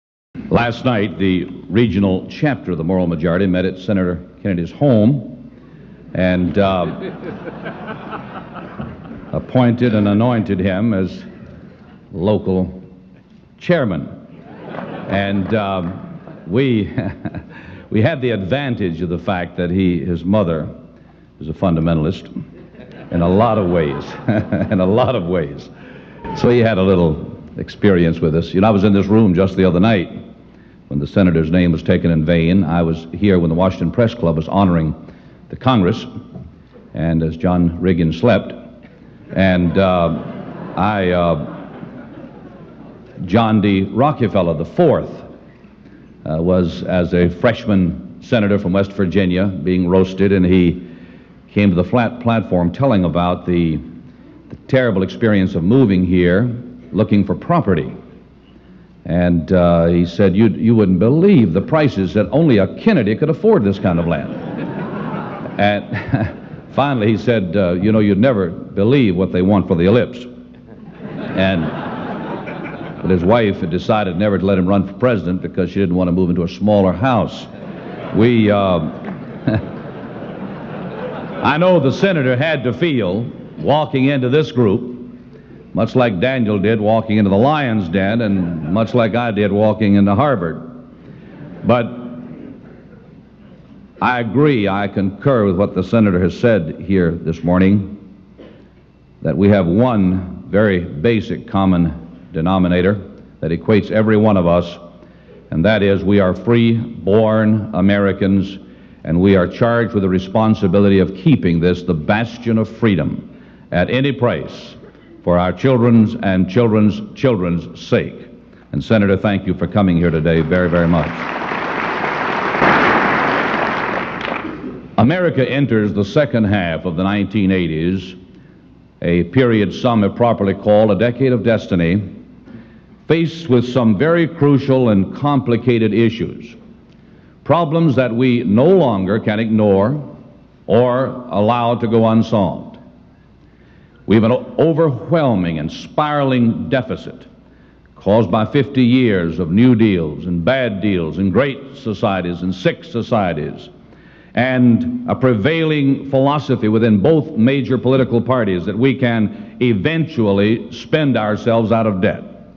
Rev. Jerry Falwell Addresses American Policy on South Africa
Rev. Jerry Falwell speaks on America's Policy toward the South African Apartheid Regime. Excerpt taken from Great Speeches Volume 7 from Educational Video Group, Inc. available